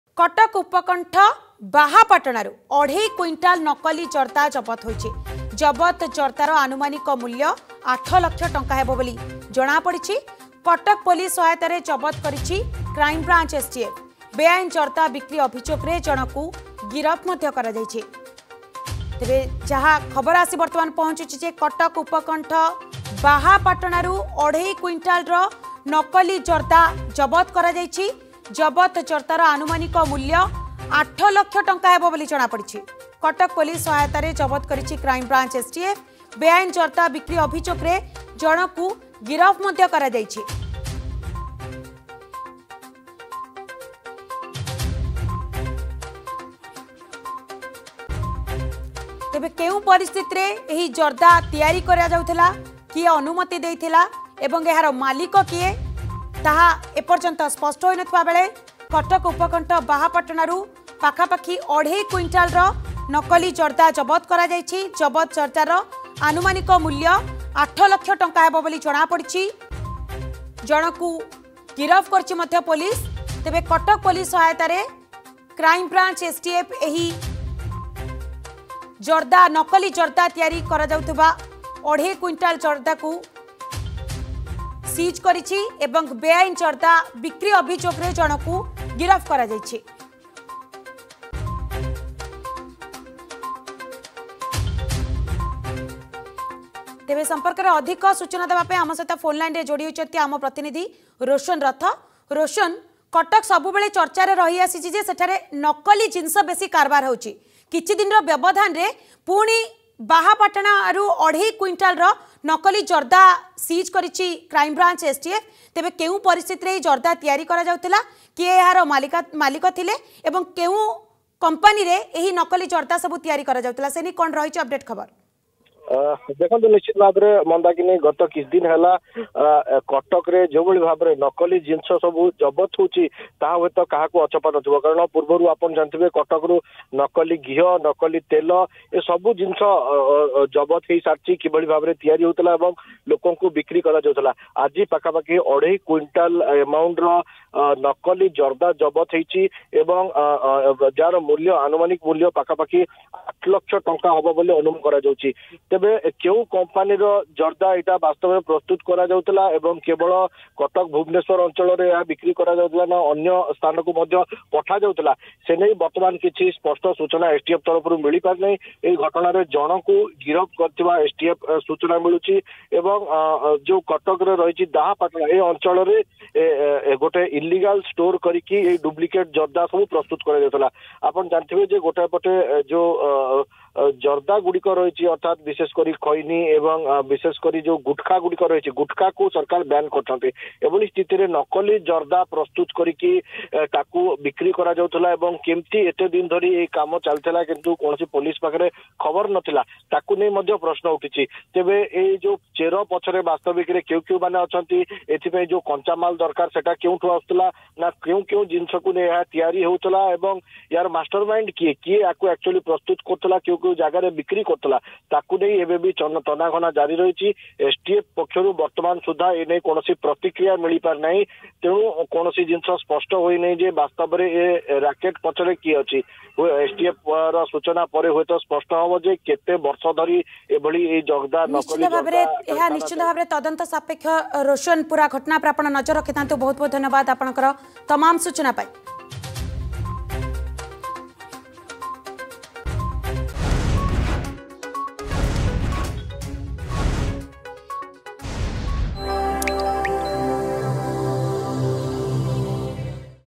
୮ ଲଷ ଟଙ୍କାର ନକଲି ଜର୍ଦା ଜବତ
କଟକ ଉପକଣ୍ଠ ଦାହାପାଟଣାରୁ ଅଢେଇ କୁଇଣ୍ଟଲ ନକଲି ଜର୍ଦା ଜବତ । ଜବତ ଜର୍ଦାର ଆନୁମାନିକ ମୂଲ୍ୟ ୮ ଲଷ ଟଙ୍କା । କଟକ ପୋଲିସ ସହାୟତାରେ ଜବତ କରିଛି କ୍ରାଇମବ୍ରାଞ୍ଚ ଏସଟିଏଫ ।